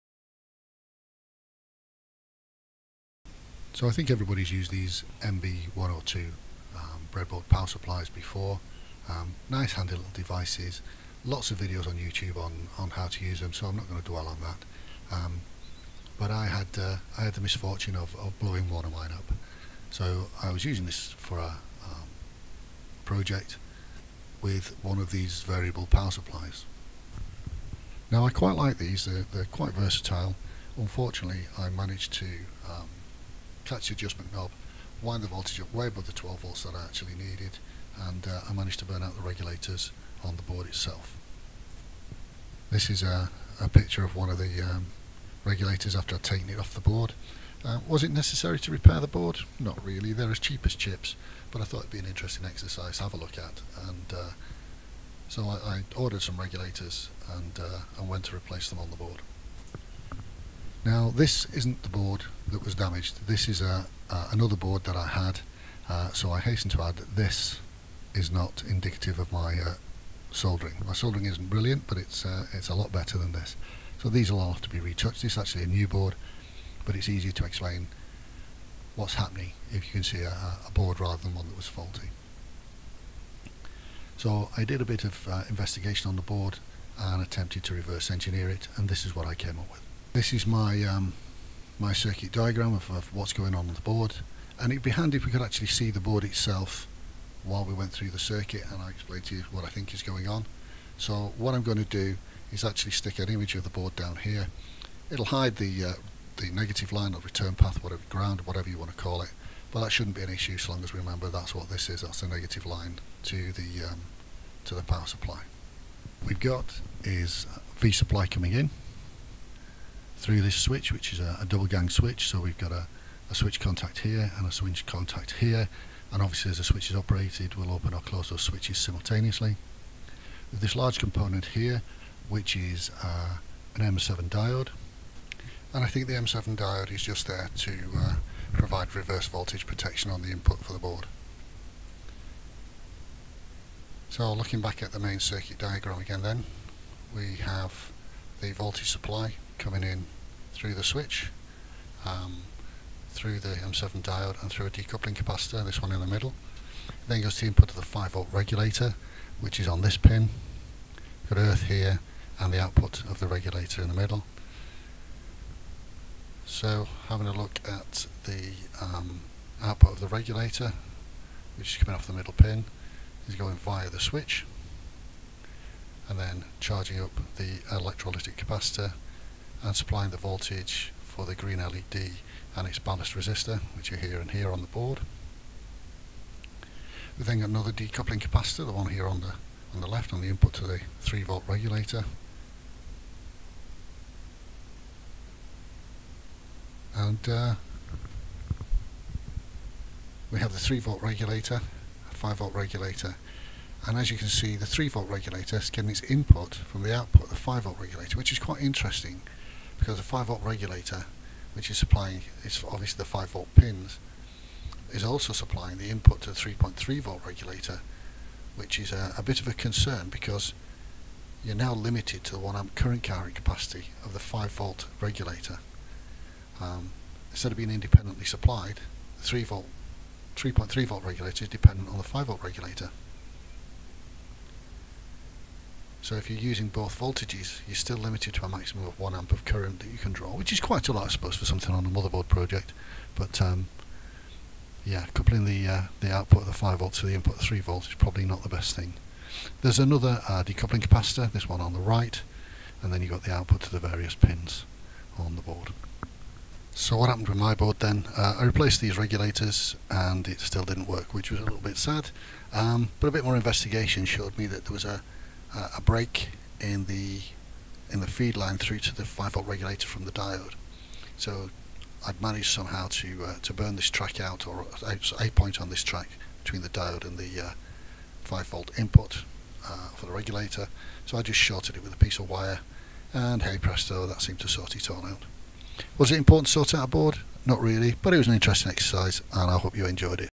ET#12. MB102 Breadboard PSU - Fail, fix, Circuit analysis / Diagram, Power Supply, Limitations.
A quick investigation into the popular MB102 breadboard power supply.
Circuit analysis revealing an interesting (and limiting) quirk in the design of this particular board.  Two blown regulators replaced but failed to fix the board - another simple solution found.
Was it an economy to fix the board - probably not.
Was it interesting?  I thought so, I hope you do too.

Since publishing this I also had a think about the little quirk I mention in the video.  I guess you could turn it to your advantage by using the USB socket as a voltage input to get both voltage levels (at low USB current levels) out of the board.  Just a thought!